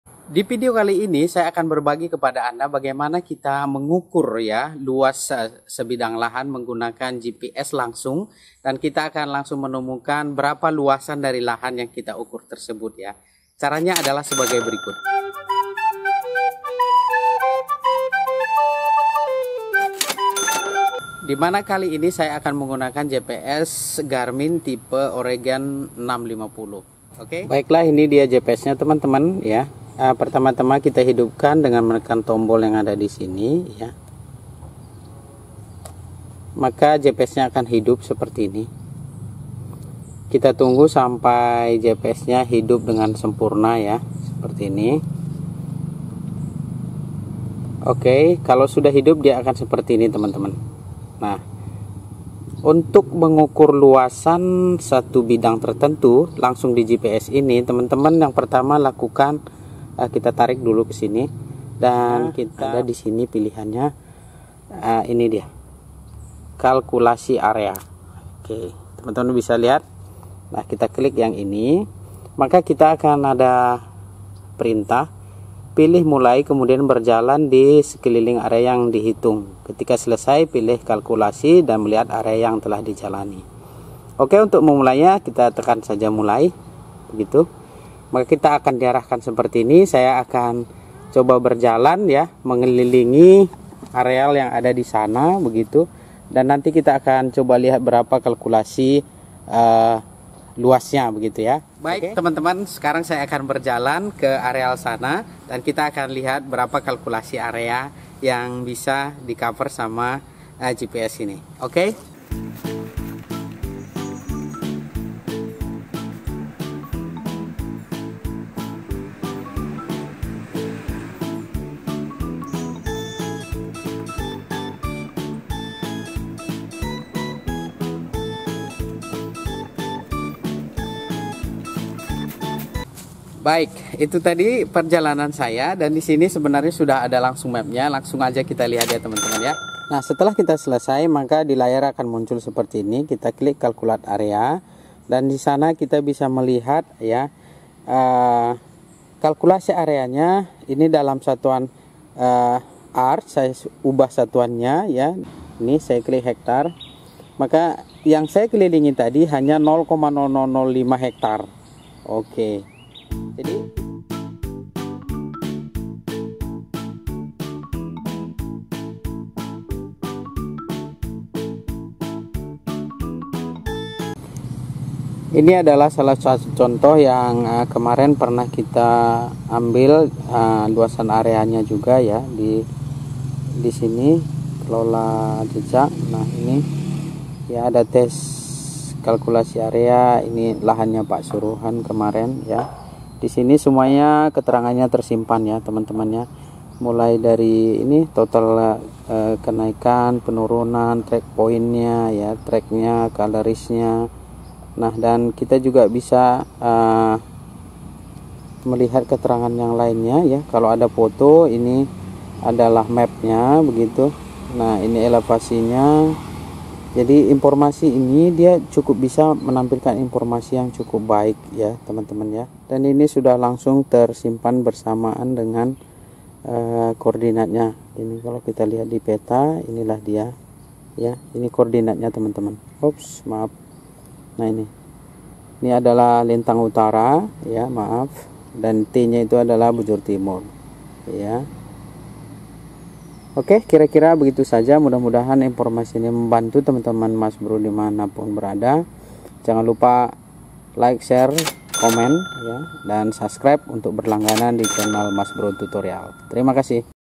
CARA MENGHITUNG LUAS LANGSUNG DI GPS GARMIN OREGON 650
Video tutorial cara menghitung luas area/lahan menggunakan gps garmin oregon 650. Cara/panduan sederhana penggunaan gps garmin oregon 650. Menghitung luas tanah dengan koordinat gps